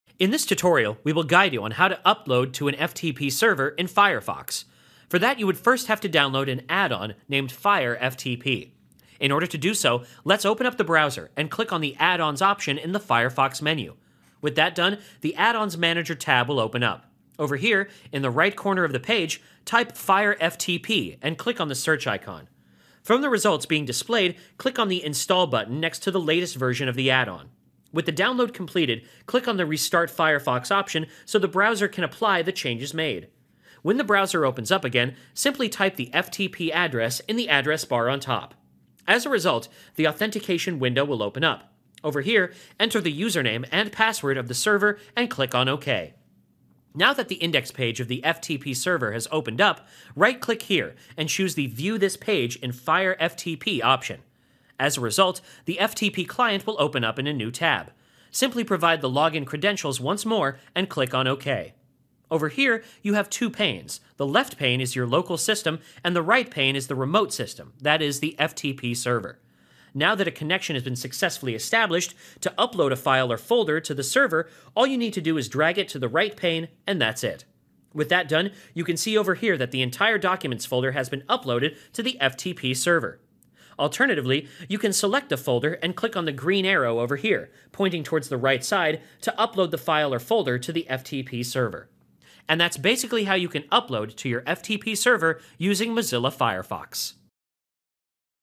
How to Upload to FTP in Firefox
In this tutorial we will guide you on how to upload to ftp in Firefox.

Follow the tutorial to learn how to upload files and folders to the FTP server in Firefox by transferring them in it easily. For that, you would first have to download an add-on for Firefox named FireFTP. Folders can be easily transferred to FTP in Firefox to learn which follow the tutorial given below.

Step 1 -- Select the Add-ons option

First of all, let's open up the browser and click on the Add-ons option in the Firefox menu.

Step 2 -- Search for fireftp

With that done, the Add-ons Manager tab will open up. Over here, in the right corner of the page, type fireftp and click on the search icon.

Step 3 -- Select the install button

From the results being displayed here, click on the install button next to the latest version of the add-on.

Step 4 -- Restart Firefox

With the download completed, click on the Restart Firefox option so that the browser can apply the changes made.

Step 5 -- Enter the address

When the browser opens up again, simply type the ftp address in the address bar on top.

As a result, the authentication window will open up. Over here, enter the username and password of the server and click on Ok.

Step 6 -- View page in FireFTP

Now that the index page of the FTP Server has opened up, right click here and choose the View this Page in FireFTP option.

Step 7 -- Enter login details

As a result, the ftp client will open up in a new tab. Simply provide the login credentials once more and click on ok.

Step 8 -- Drag folder to upload

Over here, you have two panes, the left pane is your local system and the right pane is the remote system, that is, the ftp in Firefox.

Now that a connection has been successfully established, to upload a file or folder to the server all you need to do is to drag it to the right pane and that's it.

With that done, you can see over here that the entire documents folder has been uploaded to the ftp server.

Step 9 -- Upload folder

Alternatively, you can select a folder and click on the green arrow over here, pointing towards the right side, to upload the file or folder to the Ftp server.

And that's basically what you need to learn how to upload to FTP in Firefox.